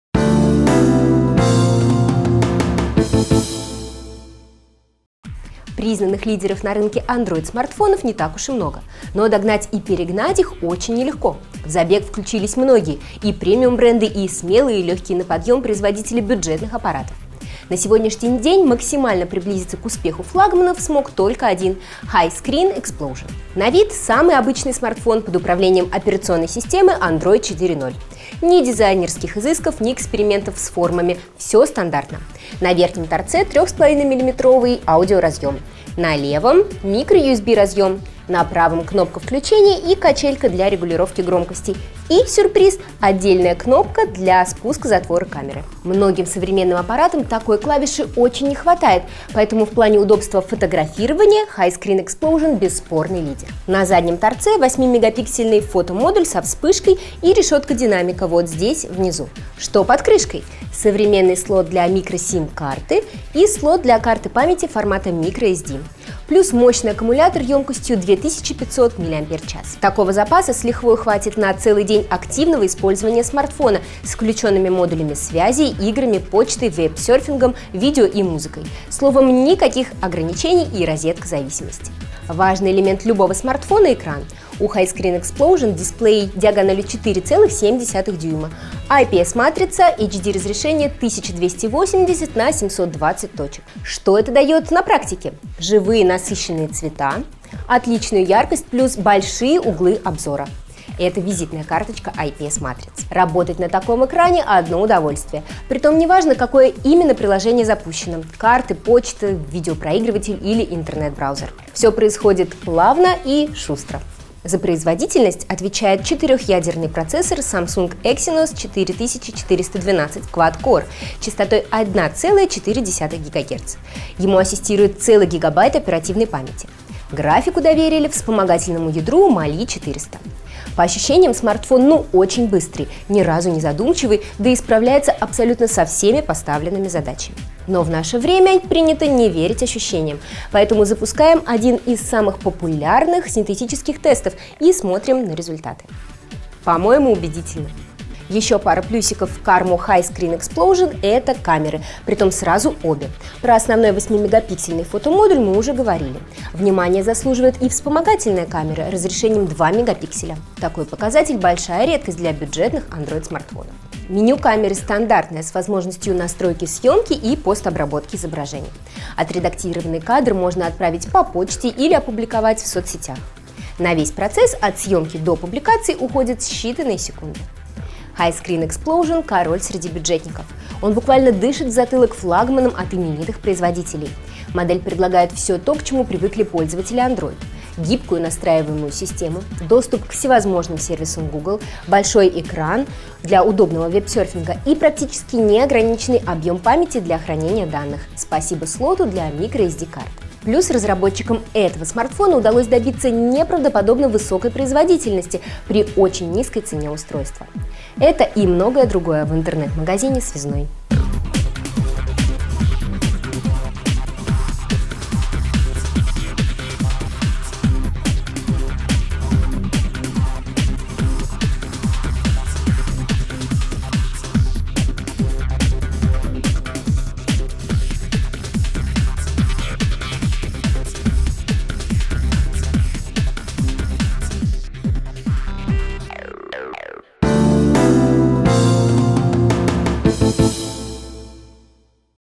Обзор смартфона Highscreen Explosion
Сегодня речь пойдет о флагмане модельного ряда Highscreen -- стильном смартфоне с мощнейшей «начинкой». Четырехъядерный процессор Samsung Exynos 4412 на 1,4 ГГц максимально расширяет границы его использования. Наслаждаться разнообразным мультимедийным контентом тем более приятно на большом  контрастном дисплее Highscreen Explosion разрешением 1280x720 точек... Рассказывать можно долго, лучше посмотрим!